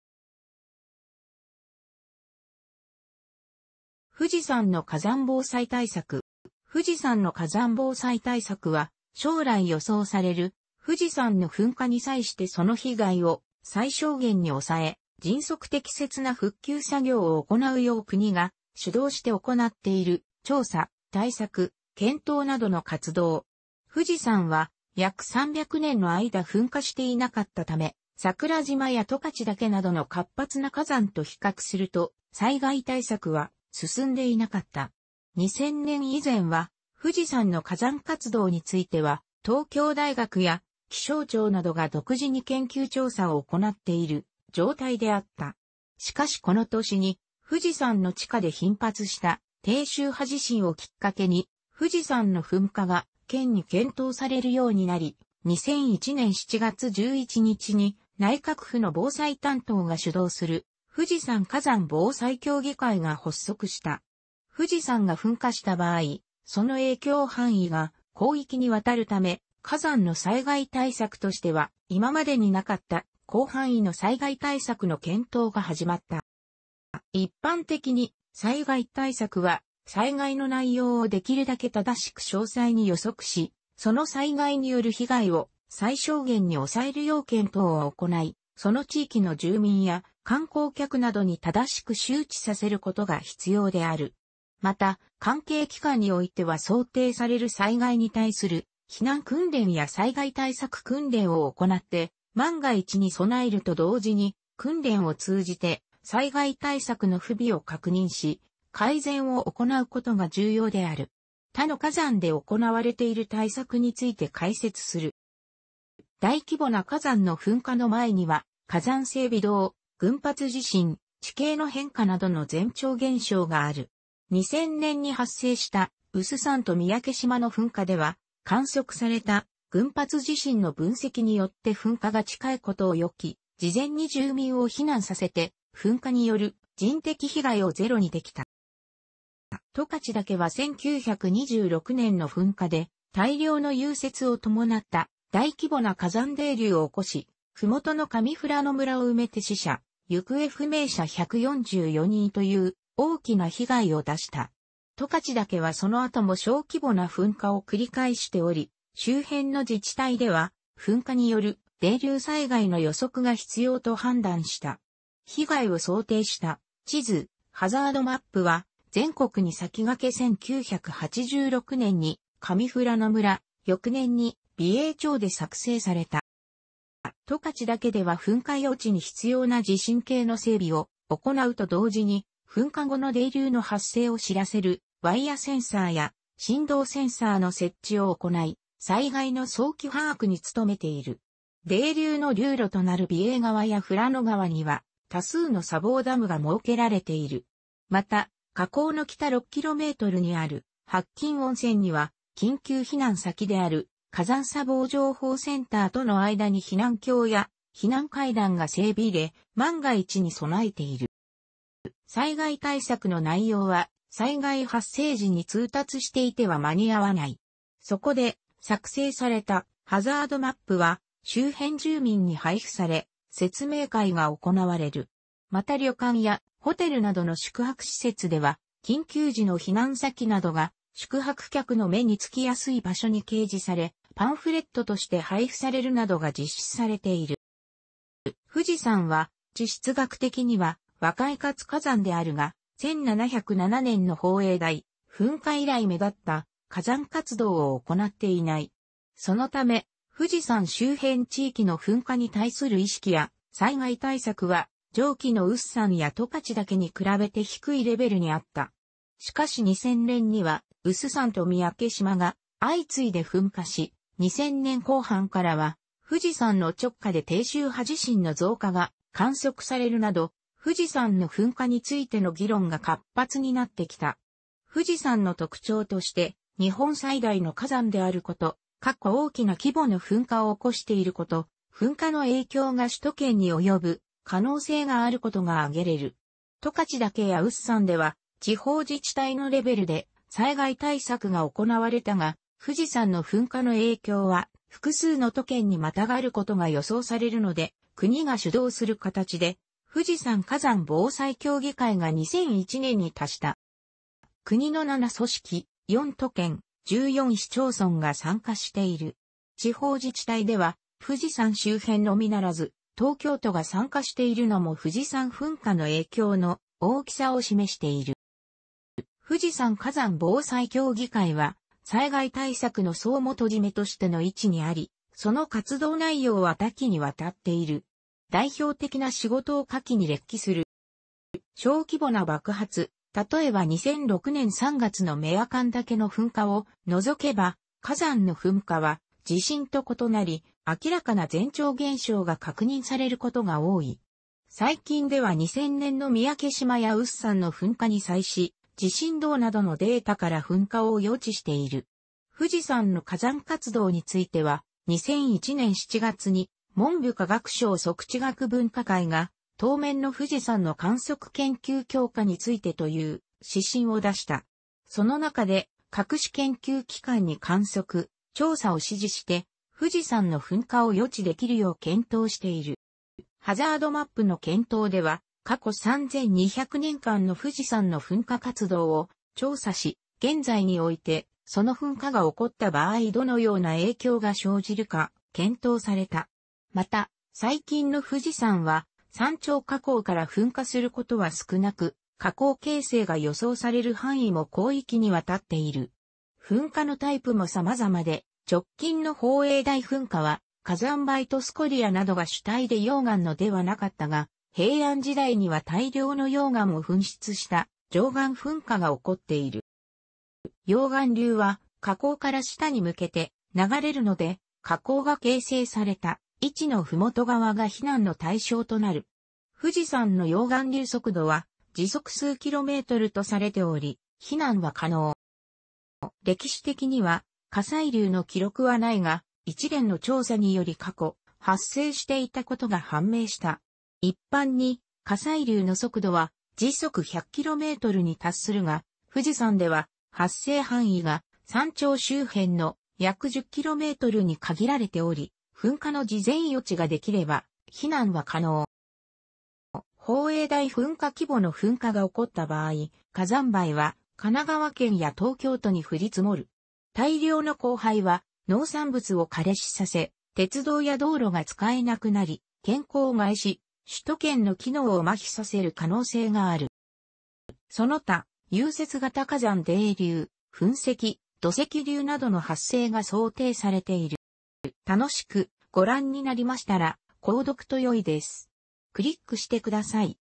富士山の火山防災対策
富士山の火山防災対策（ふじさんのかざんぼうさいたいさく）は、将来予想される富士山の噴火に際してその被害を最小限に抑え、迅速適切な復旧作業を行うよう国が主導して行っている、調査・対策・検討などの活動。富士山は約300年の間噴火していなかったため、桜島や十勝岳などの活発な火山と比較すると災害対策は進んでいなかった。2000年以前は、富士山の火山活動については東京大学や気象庁などが独自に研究調査を行っている状態であった。しかしこの年に富士山の地下で頻発した低周波地震をきっかけに富士山の噴火が真剣に検討されるようになり、2001年7月11日に内閣府の防災担当が主導する富士山火山防災協議会が発足した。富士山が噴火した場合、その影響範囲が広域にわたるため、火山の災害対策としては今までになかった広範囲の災害対策の検討が始まった。

一般的に災害対策は、災害の内容をできるだけ正しく詳細に予測し、その災害による被害を最小限に抑えるよう検討を行い、その地域の住民や観光客などに正しく周知させることが必要である。また関係機関においては想定される災害に対する避難訓練や災害対策訓練を行って万が一に備えると同時に、訓練を通じて災害対策の不備を確認し改善を行うことが重要である。他の火山で行われている対策について解説する。

大規模な火山の噴火の前には火山性微動、群発地震、地形の変化などの前兆現象がある。2000年に発生した有珠山と三宅島の噴火では、観測された群発地震の分析によって噴火が近いことを予期し、事前に住民を避難させて噴火による人的被害をゼロにできた。

十勝岳は1926年の噴火で大量の融雪を伴った大規模な火山泥流を起こし、麓の上富良野村を埋めて死者・行方不明者144人という大きな被害を出した。十勝岳はその後も小規模な噴火を繰り返しており、周辺の自治体では噴火による泥流災害の予測が必要と判断した。被害を想定した地図（ハザードマップ）は全国に先駆け1986年に上富良野村、翌年に美瑛町で作成された。

十勝岳では噴火予知に必要な地震計の整備を行うと同時に、噴火後の泥流の発生を知らせる「ワイヤセンサー」や「振動センサー」の設置を行い、災害の早期把握に努めている。泥流の流路となる美瑛川や富良野川には多数の砂防ダムが設けられている。また火口の北6kmにある白金温泉には、緊急避難先である「火山砂防情報センター」との間に避難橋や避難階段が整備され万が一に備えている。

災害対策の内容は、災害発生時に通達していては間に合わない。そこで、作成されたハザードマップは周辺住民に配布され、説明会が行われる。また旅館やホテルなどの宿泊施設では、緊急時の避難先などが宿泊客の目に付きやすい場所に掲示され、パンフレットとして配布されるなどが実施されている。

富士山は地質学的には若い活火山であるが、1707年の宝永大噴火以来目だった火山活動を行っていない。そのため、富士山周辺地域の噴火に対する意識や災害対策は、上記の有珠山や十勝岳に比べて低いレベルにあった。しかし2000年には有珠山と三宅島が相次いで噴火し、2000年後半からは富士山の直下で低周波地震の増加が観測されるなど、富士山の噴火についての議論が活発になってきた。富士山の特徴として日本最大の火山であること、過去大きな規模の噴火を起こしていること、噴火の影響が首都圏に及ぶ可能性があることが挙げられる。十勝岳や有珠山では地方自治体のレベルで災害対策が行われたが、富士山の噴火の影響は複数の都県にまたがることが予想されるので、国が主導する形で富士山火山防災協議会が2001年に発足した。

国の7組織、4都県、14市町村が参加している。地方自治体では富士山周辺のみならず東京都が参加しているのも富士山噴火の影響の大きさを示している。

富士山火山防災協議会は、災害対策の総元締めとしての位置にあり、その活動内容は多岐にわたっている。代表的な仕事を下記に列記する。

小規模な爆発（例えば2006年3月の雌阿寒岳の噴火）を除けば、火山の噴火は地震と異なり、明らかな前兆現象が確認されることが多い。最近では2000年の三宅島や有珠山の噴火に際し、地震動などのデータから噴火を予知している。富士山の火山活動については、2001年7月に文部科学省測地学分科会が「当面の富士山の観測研究の強化について」という指針を出した。その中で各種研究機関に観測・調査を指示して、富士山の噴火を予知できるよう検討している。

ハザードマップの検討では過去3200...